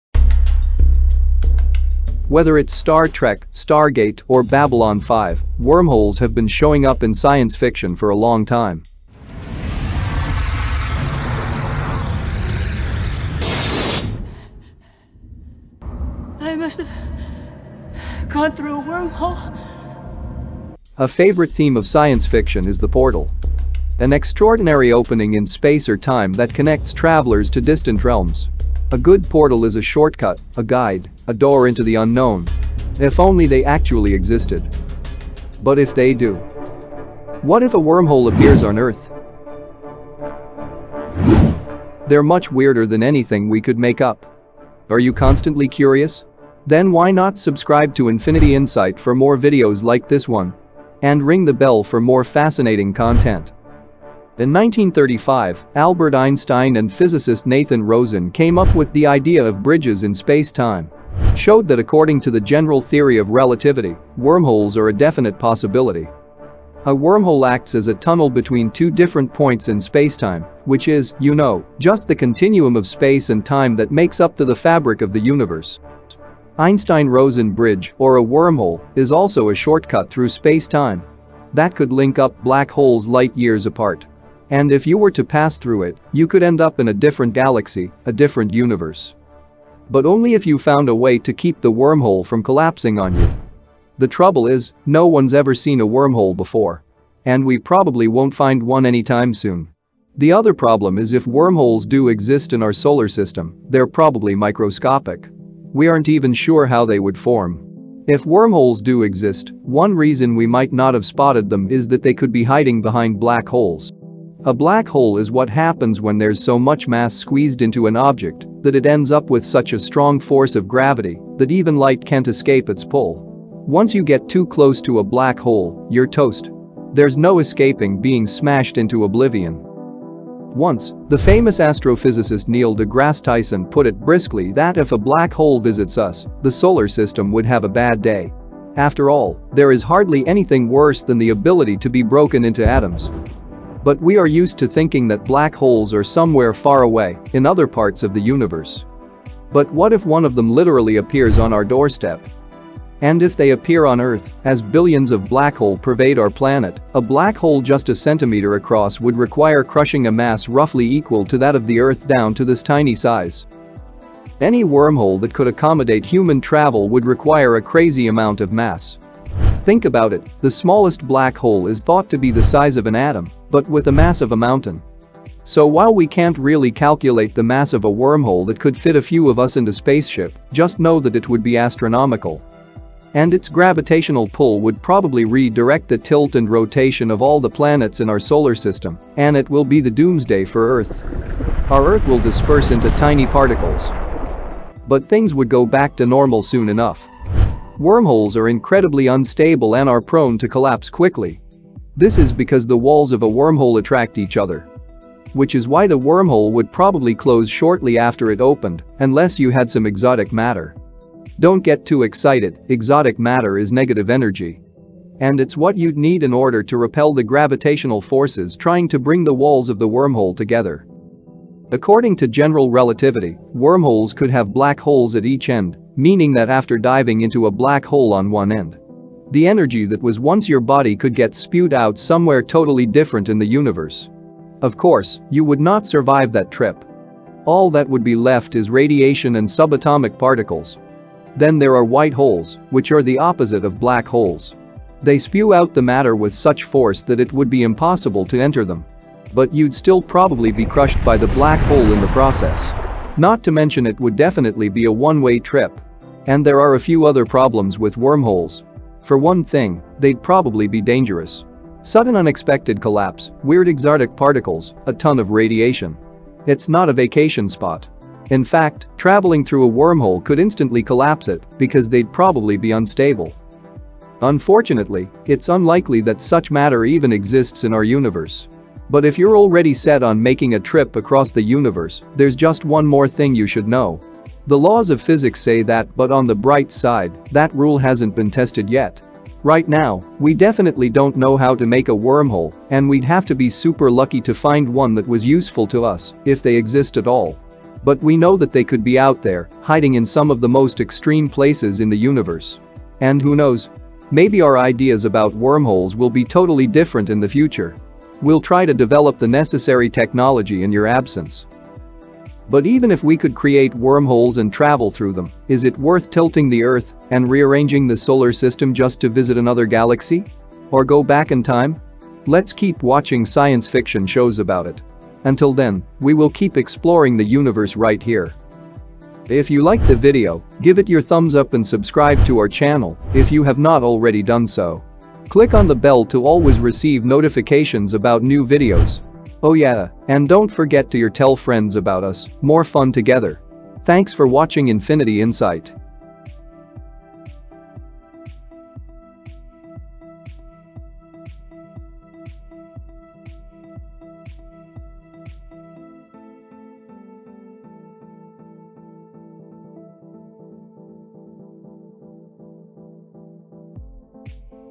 What If a Wormhole Appears On Earth?
A wormhole is a theoretical passage through space-time that could create shortcuts for
long journeys across the universe. In this video, we talk about
What if Wormhole Do exist and what if they appear on earth?